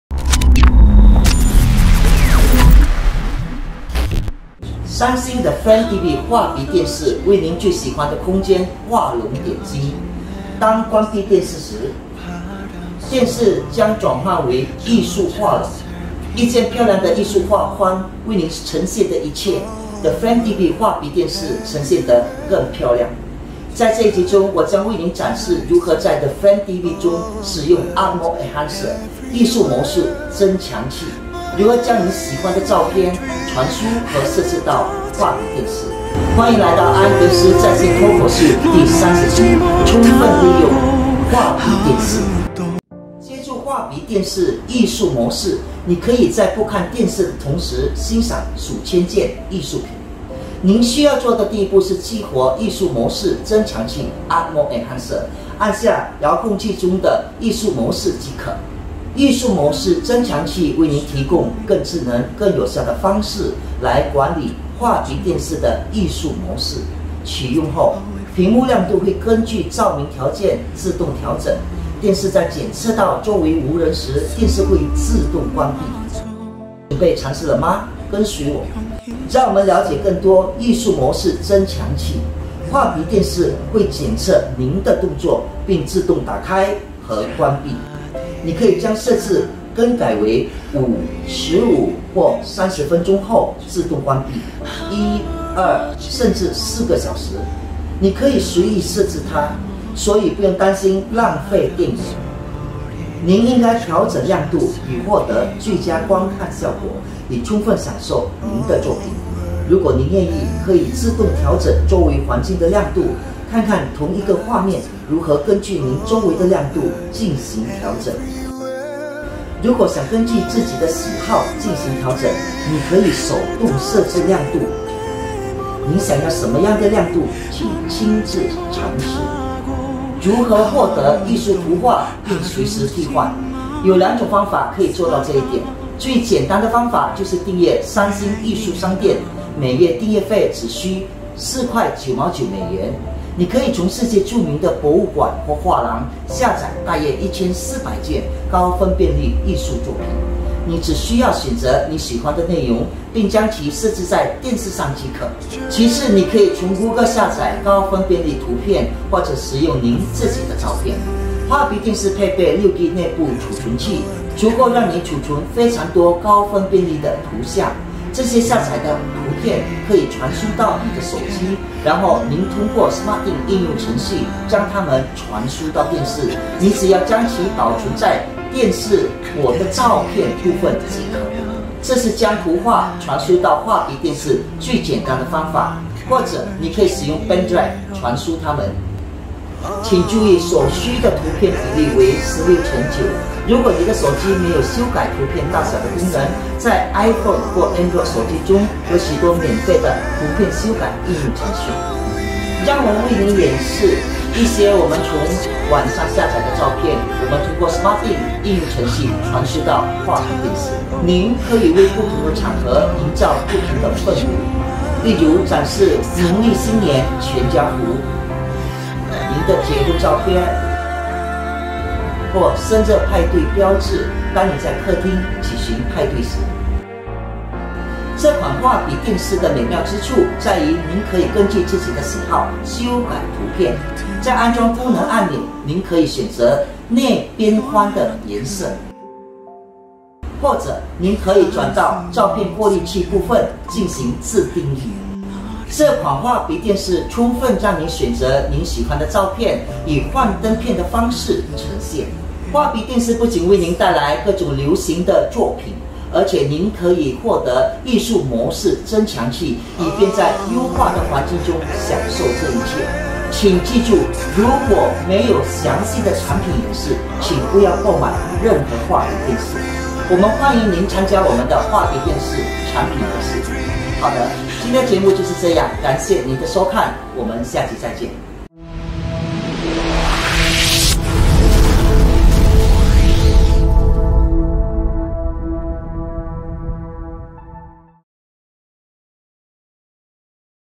充分利用画框电视The Frame TV
在这一集中，我将向您展示如何在 The Frame TV 中使用 Art Mode Enhancer艺术模式增强器，如何将您喜欢的照片传输和设置到 The Frame TV